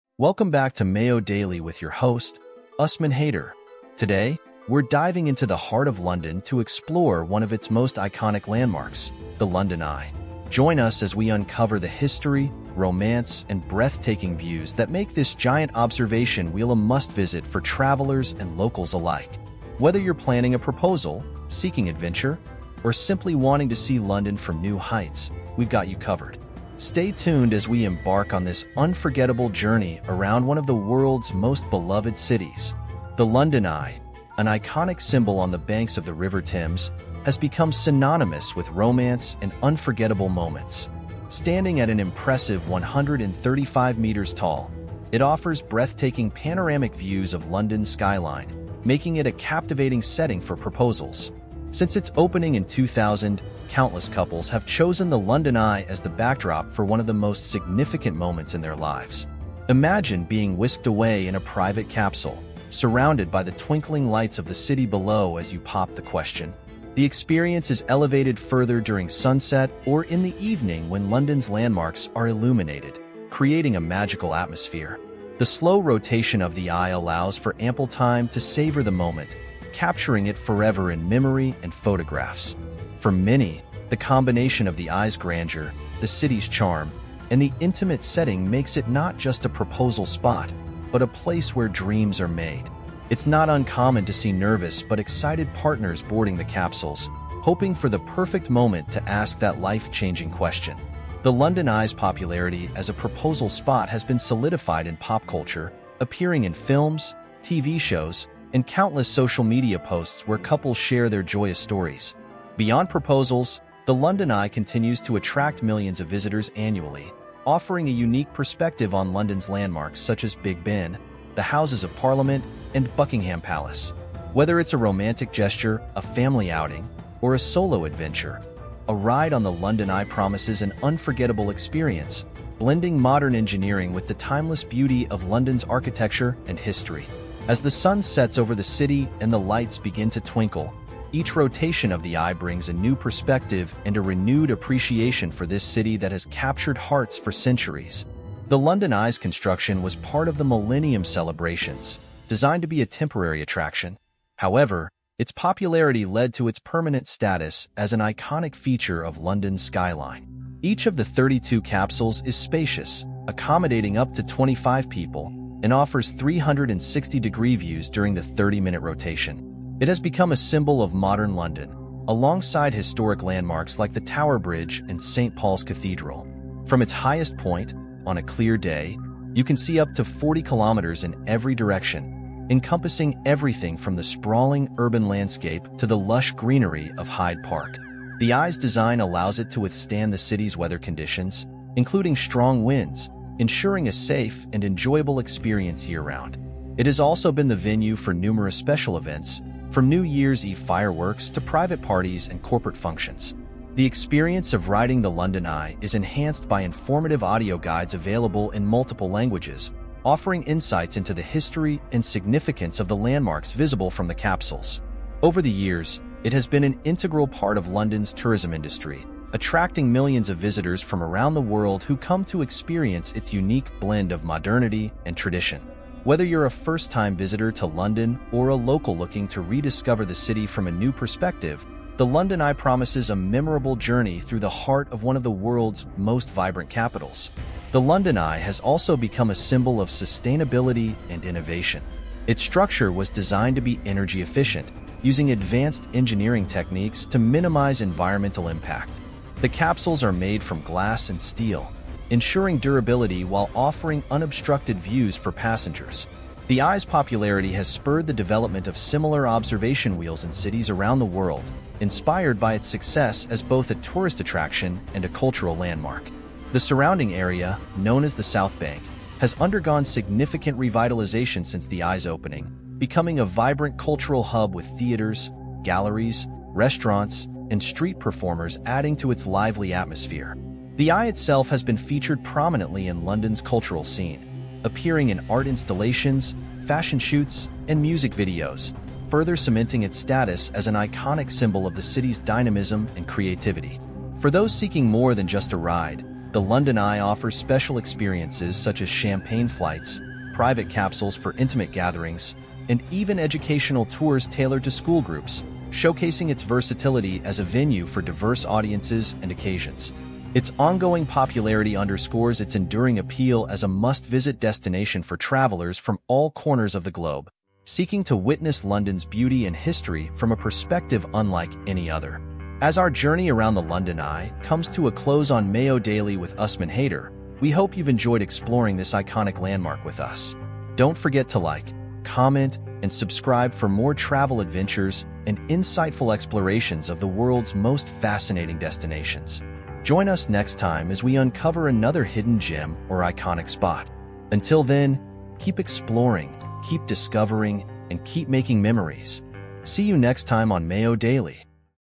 London Eye: World's Most Epic Proposal Spot?
Welcome back to Mayo Daily with your host, Usman Haider! Today, we're diving into the heart of London to explore one of its most iconic landmarks, the London Eye. Join us as we uncover the history, romance, and breathtaking views that make this giant observation wheel a must-visit for travelers and locals alike. Whether you're planning a proposal, seeking adventure, or simply wanting to see London from new heights, we've got you covered.

The London Eye, an iconic symbol on the banks of the River Thames, has become synonymous with romance and unforgettable moments. Standing at an impressive 135 meters tall, it offers breathtaking panoramic views of London's skyline, making it a captivating setting for proposals. Since its opening in 2000, countless couples have chosen the London Eye as the backdrop for one of the most significant moments in their lives. Imagine being whisked away in a private capsule, surrounded by the twinkling lights of the city below as you pop the question. The experience is elevated further during sunset or in the evening when London's landmarks are illuminated, creating a magical atmosphere. The slow rotation of the Eye allows for ample time to savor the moment, capturing it forever in memory and photographs. For many, the combination of the Eye's grandeur, the city's charm, and the intimate setting makes it not just a proposal spot, but a place where dreams are made. It's not uncommon to see nervous but excited partners boarding the capsules, hoping for the perfect moment to ask that life-changing question. The London Eye's popularity as a proposal spot has been solidified in pop culture, appearing in films, TV shows, and countless social media posts where couples share their joyous stories. Beyond proposals, the London Eye continues to attract millions of visitors annually, offering a unique perspective on London's landmarks such as Big Ben, the Houses of Parliament, and Buckingham Palace. Whether it's a romantic gesture, a family outing, or a solo adventure, a ride on the London Eye promises an unforgettable experience, blending modern engineering with the timeless beauty of London's architecture and history. As the sun sets over the city and the lights begin to twinkle, each rotation of the Eye brings a new perspective and a renewed appreciation for this city that has captured hearts for centuries.

The London Eye's construction was part of the Millennium celebrations, designed to be a temporary attraction. However, its popularity led to its permanent status as an iconic feature of London's skyline. Each of the 32 capsules is spacious, accommodating up to 25 people, and offers 360-degree views during the 30-minute rotation. It has become a symbol of modern London, alongside historic landmarks like the Tower Bridge and St. Paul's Cathedral. From its highest point, on a clear day, you can see up to 40 kilometers in every direction, encompassing everything from the sprawling urban landscape to the lush greenery of Hyde Park. The Eye's design allows it to withstand the city's weather conditions, including strong winds, ensuring a safe and enjoyable experience year-round. It has also been the venue for numerous special events, from New Year's Eve fireworks to private parties and corporate functions. The experience of riding the London Eye is enhanced by informative audio guides available in multiple languages, offering insights into the history and significance of the landmarks visible from the capsules. Over the years, it has been an integral part of London's tourism industry, attracting millions of visitors from around the world who come to experience its unique blend of modernity and tradition. Whether you're a first-time visitor to London or a local looking to rediscover the city from a new perspective, the London Eye promises a memorable journey through the heart of one of the world's most vibrant capitals.